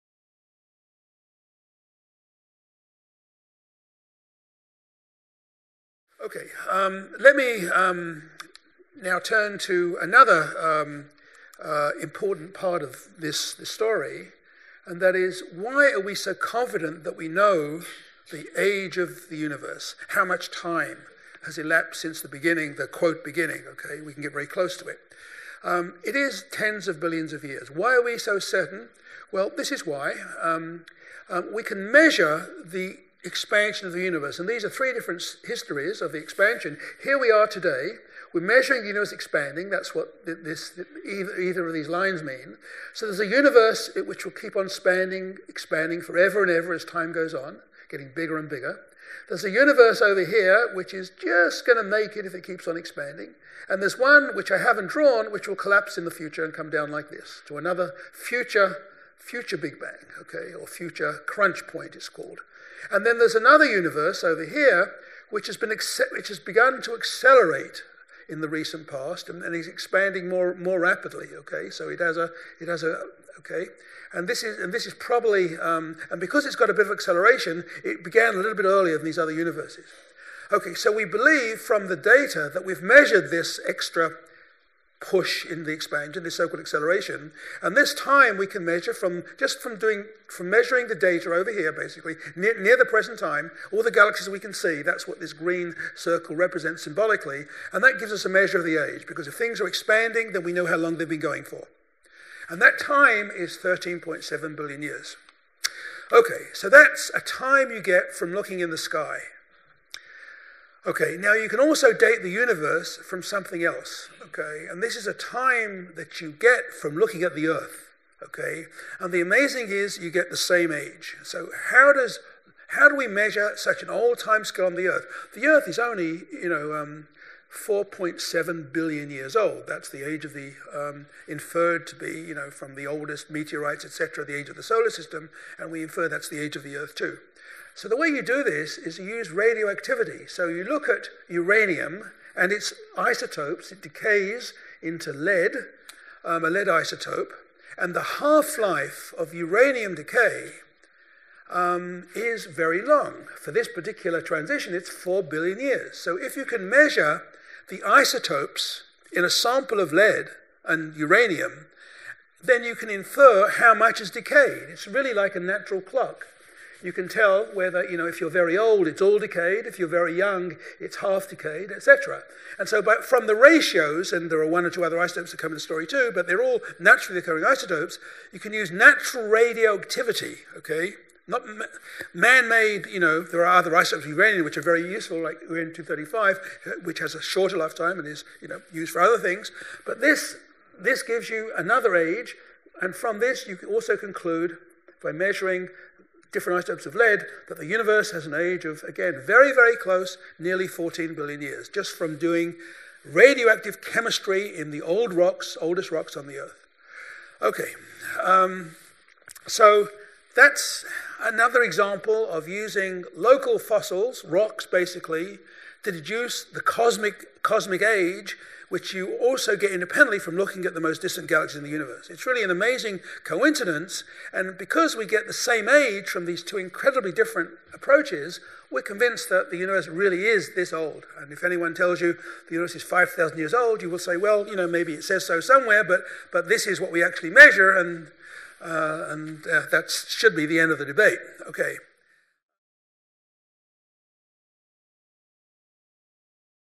The Age of the Universe - Professor Joseph Silk
Astronomers have managed to determine the age of the universe. Professor Silk explains how and why this information is useful to cosmologists.